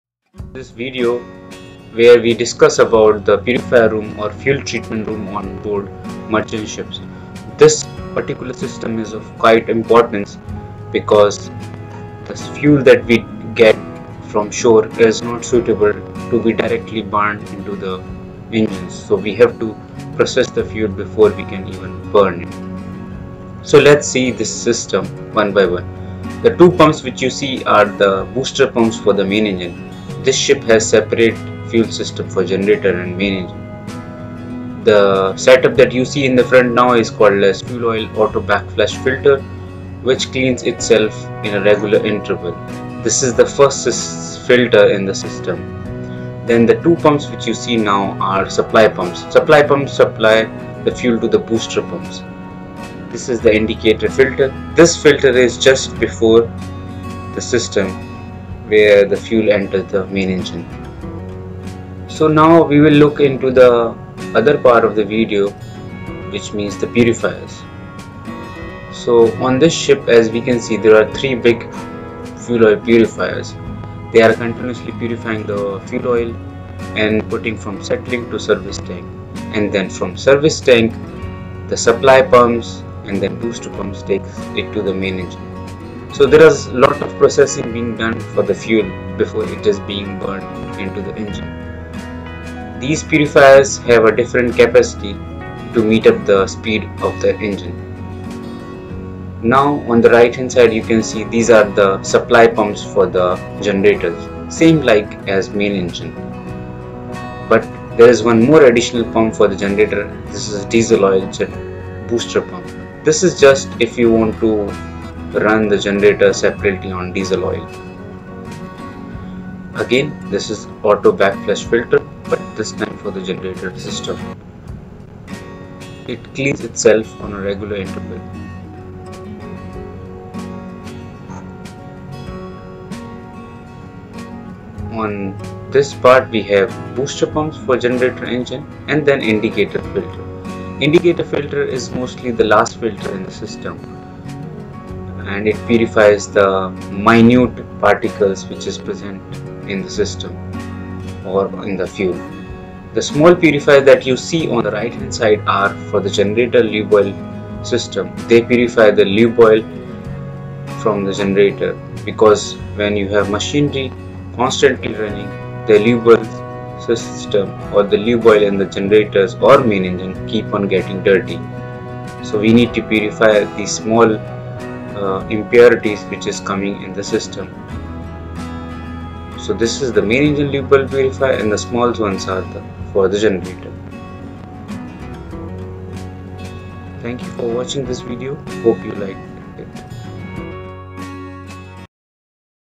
Purifier Room Tour On A Merchant Ship | Alfa Laval, Westfalia Seperators | Fuel Oil Heaters
This video shows us the fuel purification plant on ships. Fuel oil purification is a major job to be done on the ship. Why? well the ship receives oil in purified form but still it needs to be treated and purified so that excessive water and sediments can be taken out.

Otherwise, these impurities will choke the fuel lines and will create a major issue for the engines which are operating on such fuel.

Some of the equipment that are available in the Purifier Room on Ship are:
Fuel and Lube Oil Seperator
Automatic Backflush FILTERS.
Heaters for the fuel oil
Fuel oil supply and circulating pumps.